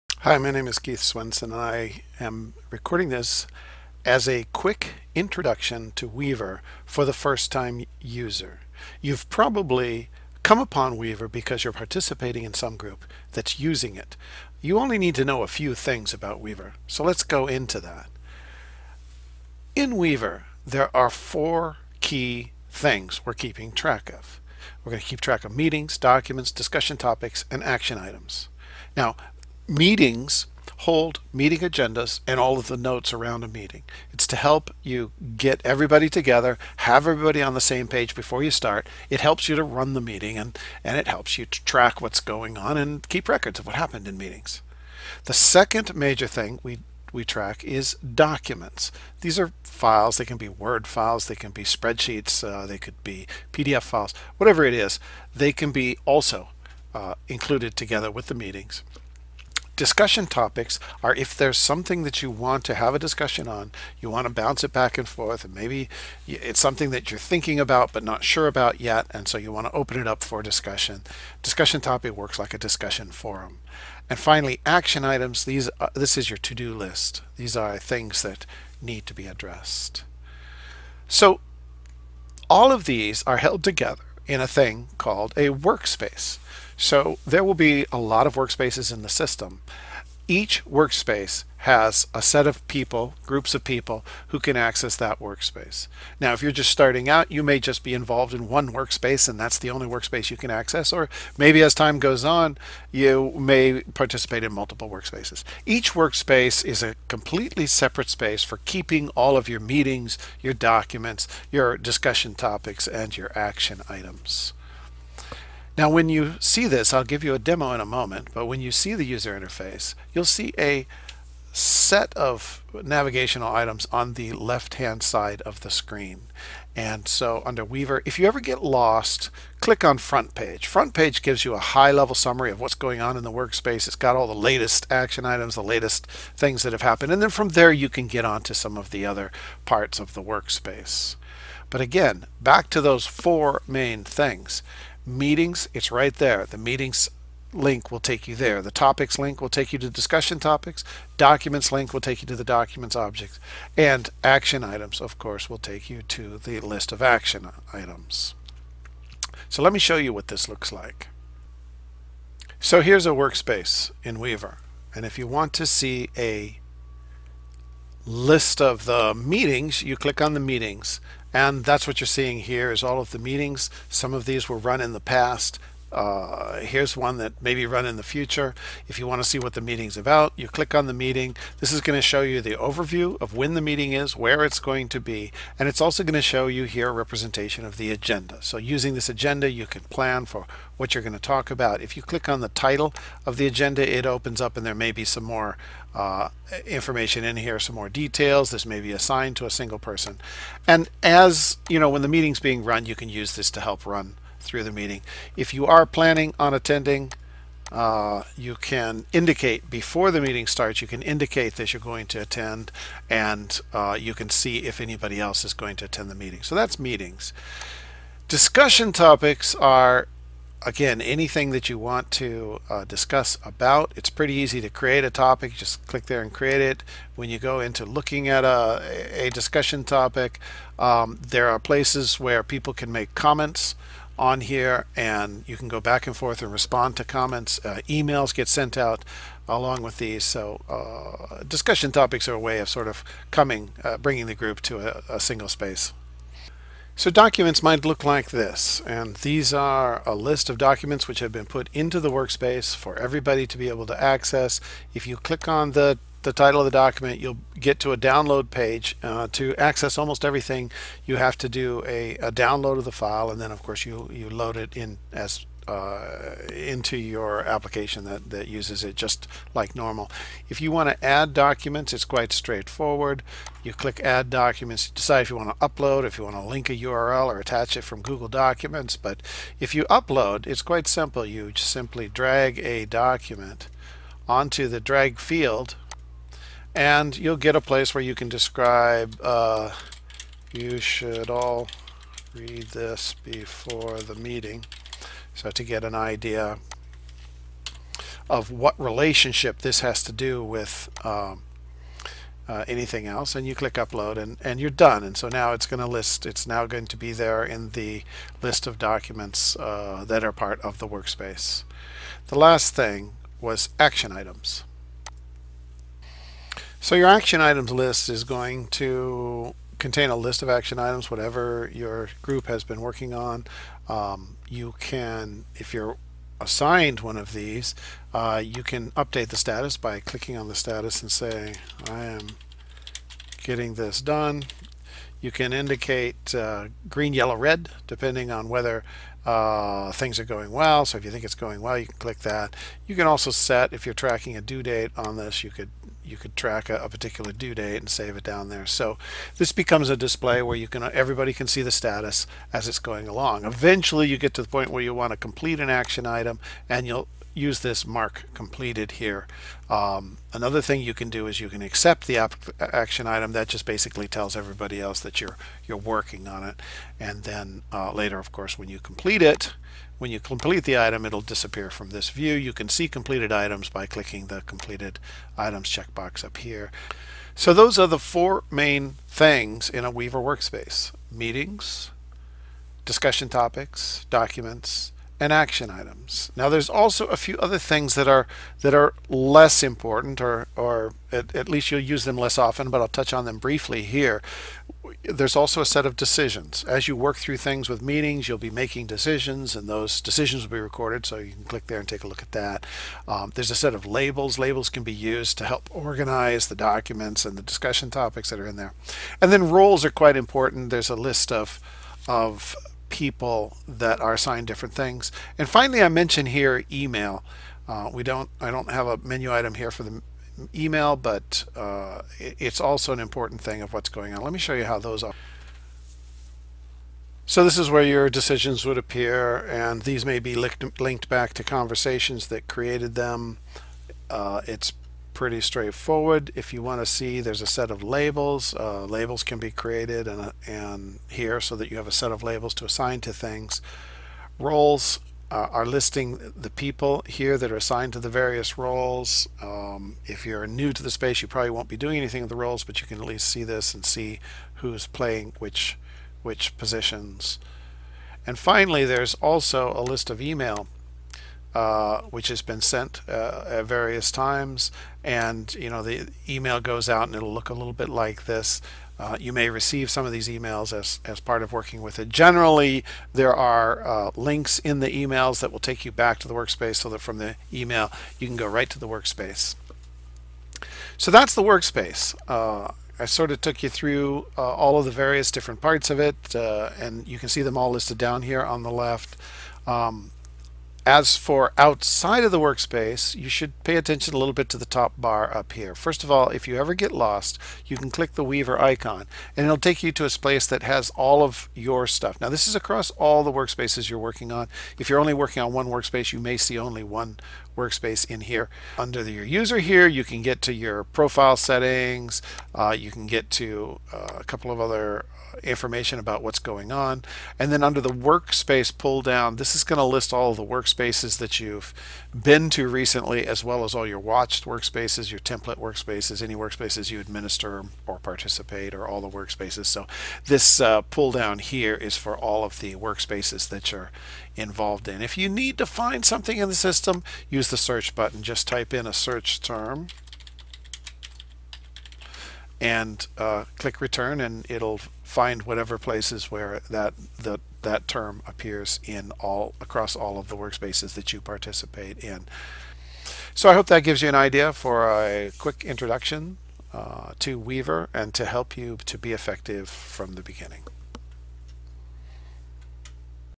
Weaver for First Time User
This is a short, very high level description of the various things that you can manipulate in Weaver and how to participate withyou group in there.